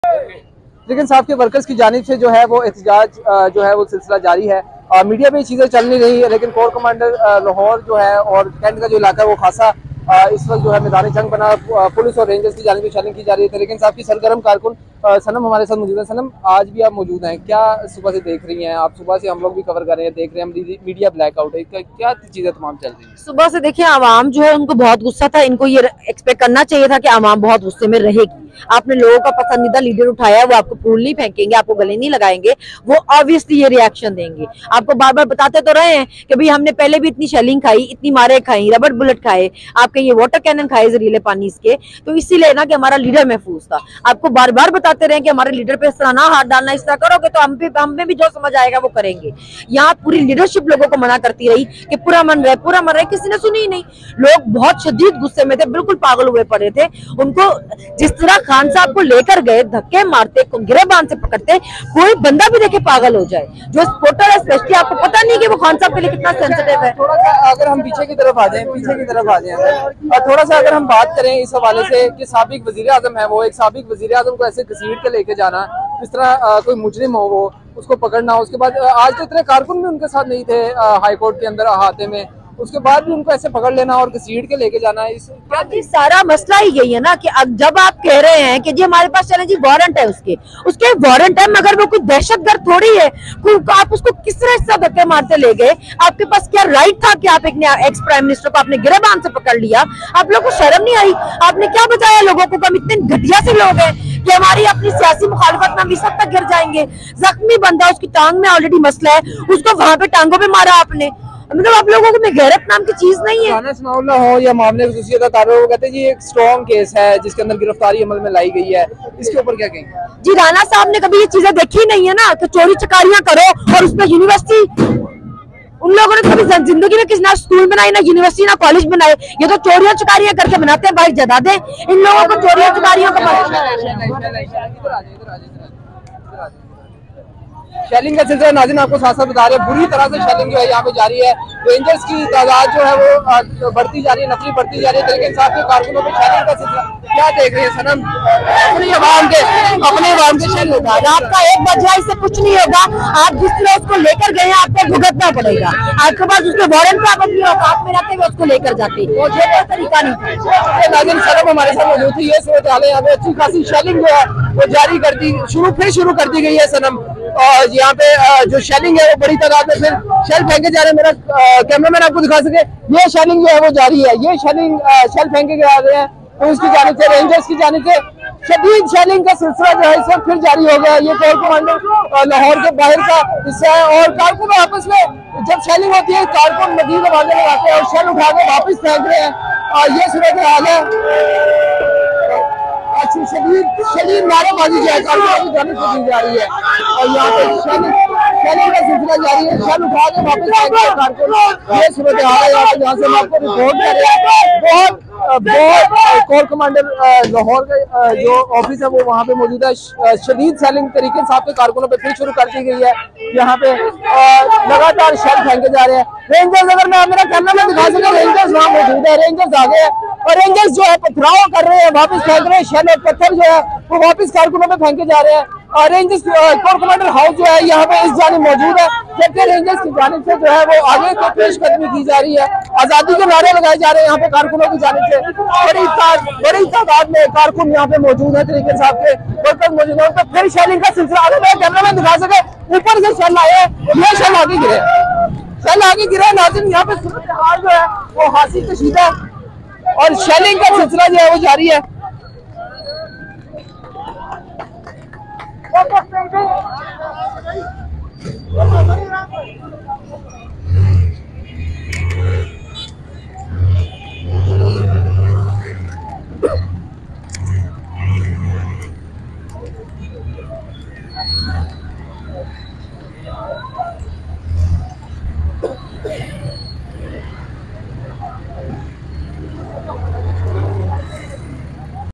Sanam Javed bashes on PDM and Generals at Core Commander House Lahore
After imran khan arrest Sanam Javed khan came into the protest at core commander house lahore and bashes general, PDM government over the arrest of Imran khan